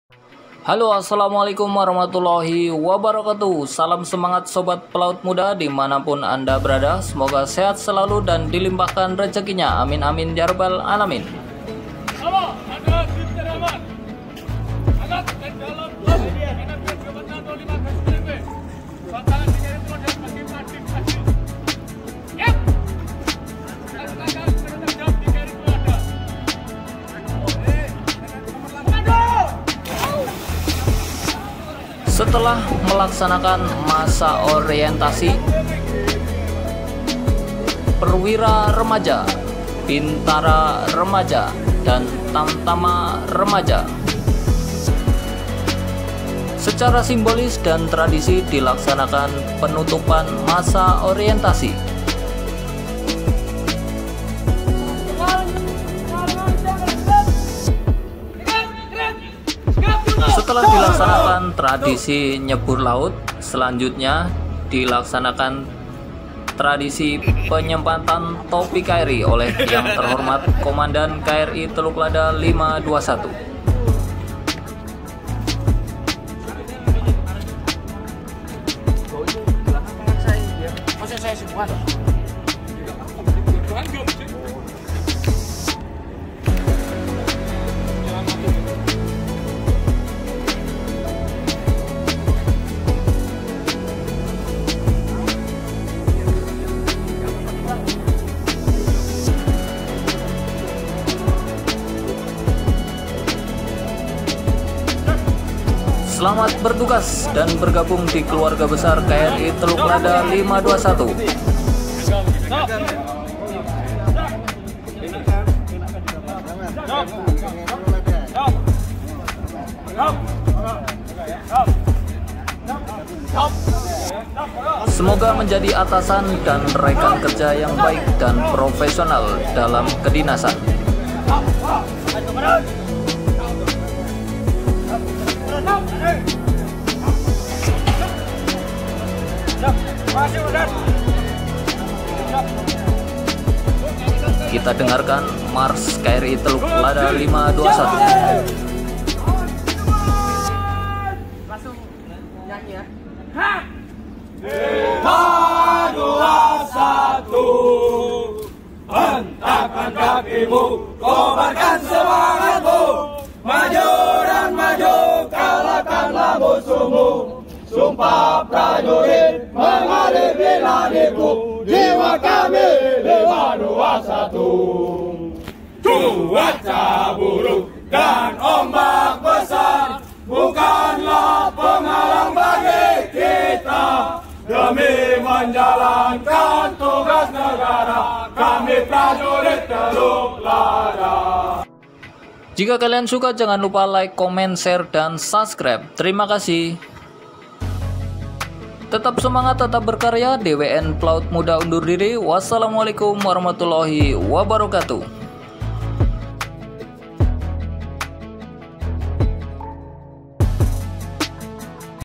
TRADISI TNI AL || PENUTUPAN MASA ORIENTASI PRAJURIT REMAJA TNI AL DI KRI TELUK LADA - 521
salam semangat sobat pelaut muda di seluruh lautan Nusantara
semoga sehat selalu dan banyak rejekinya
aamiin 🤲

setelah melaksanakan kegiatan masa orientasi
prajurit remaja TNI AL yg masuk dinas di kapal perang, dilaksanakan kegiatan tradisi penutupan masa orientasi

dengan menceburkan diri ke laut dan disematkan topi KRI oleh Yth Komandan

DWN pelaut muda

terimakasih